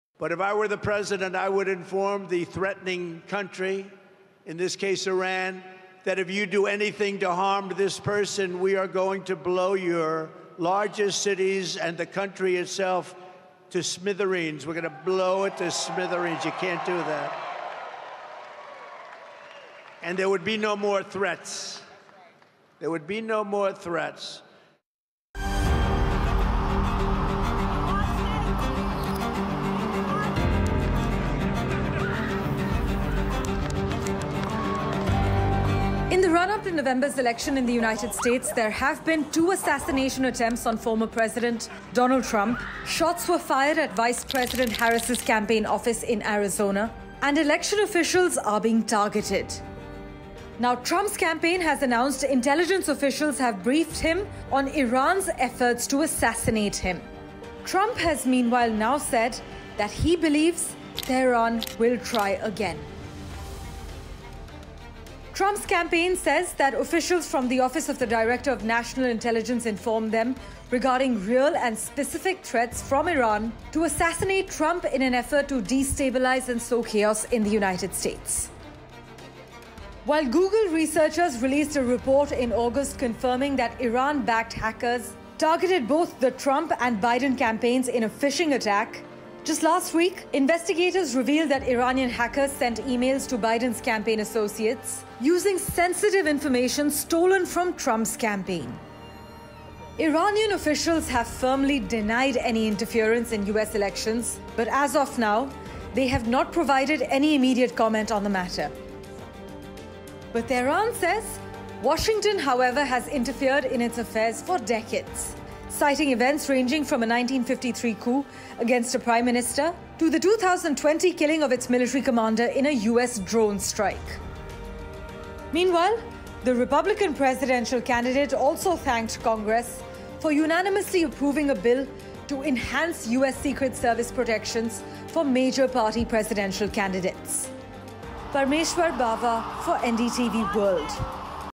Trump Latest News | "Will Blow You To Smithereens": Trump Warns Iran After Intel Flags Threat
Donald Trump has said Iran should be blown "to smithereens" if the Islamic Republic is involved in the harming of any US White House candidate or ex-president. His remarks come after US intelligence warned of threats from Tehran against the Republican's life after two apparent assassination attempts in recent months, Parmeshwar Bawa reports.